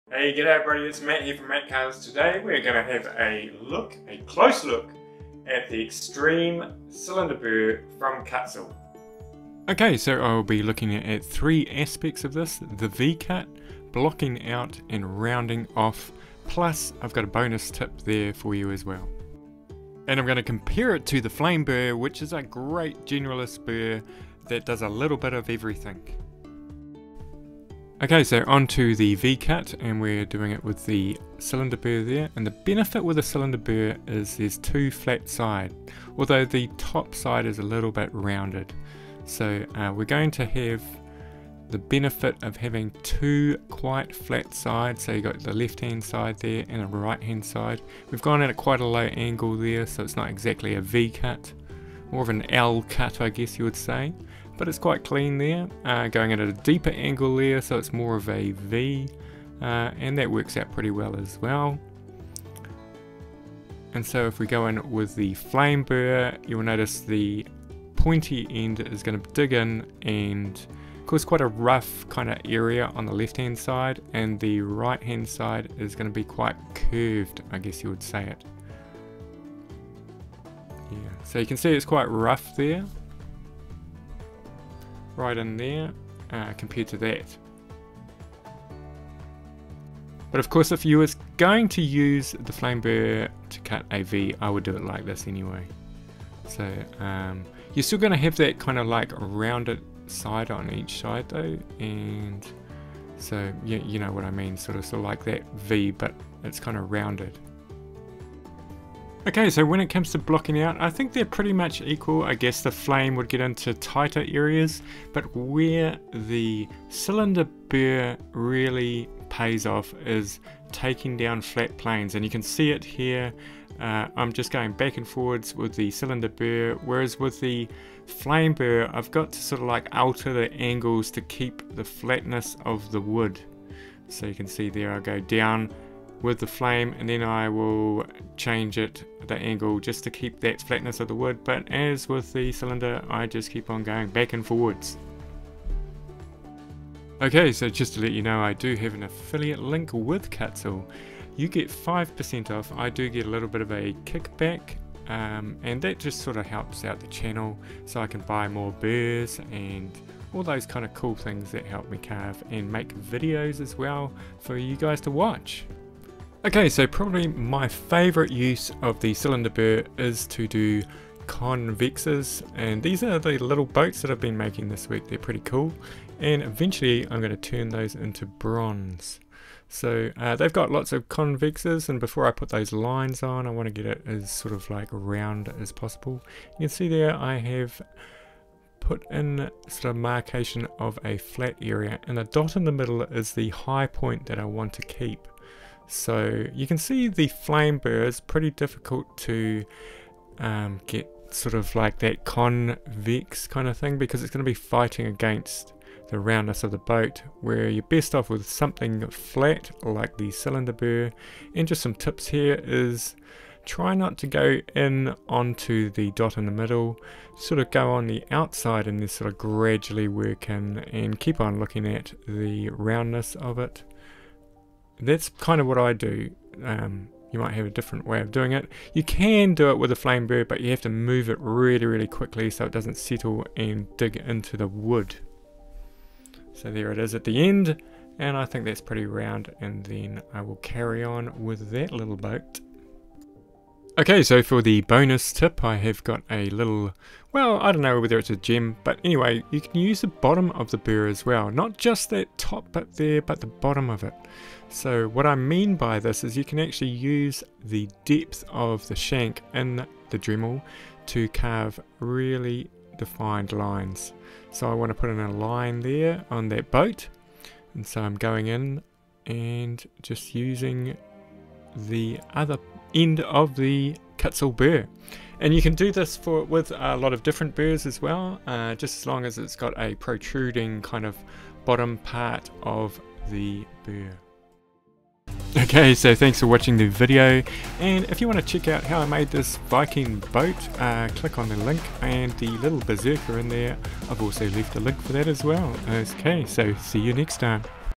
Kutzall Burrs
Tips for using Kutzall Burrs, this video concentrates on the Kutzall 1/8th Cylinder Burr. More videos to come on individual burrs.

Chapters

 0:00 Introduction to the Extreme Kutzall Cylinder burr
0:27 Extreme Kutzall Flame vs the Cylinder burr for V cuts
1:47 Blocking out with kutzall burrs
2:51 Convex carving with Kutzall Burrs
4:05 Kutzall burrs experimental method

Music supplied by Artlist